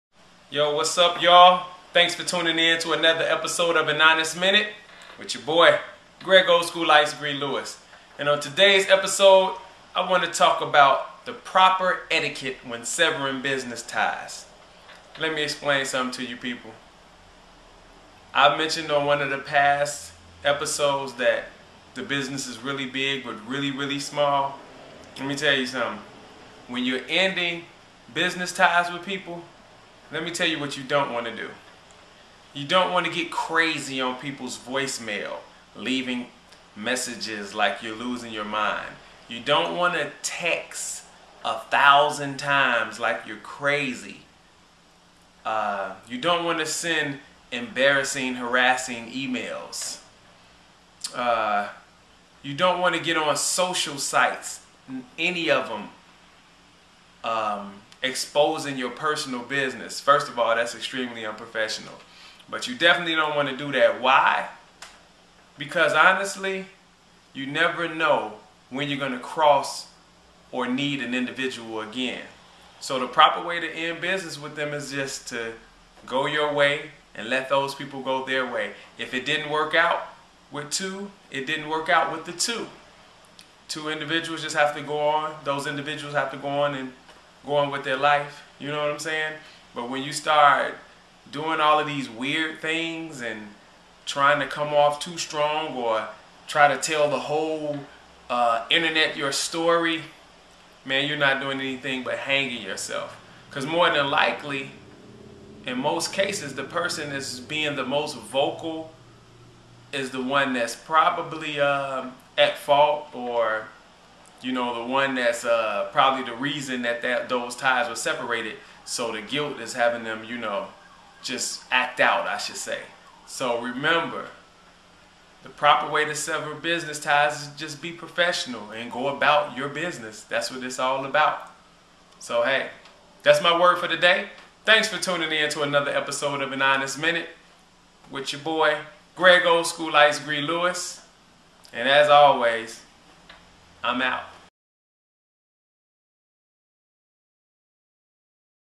An Honest Minute Episode 21
In this episode of An Honest Minute Greg "Olskool Ice-Gre" Lewis builds on the proper etiquette when severing business ties.

Twitter Follow: @Olskool_IceGre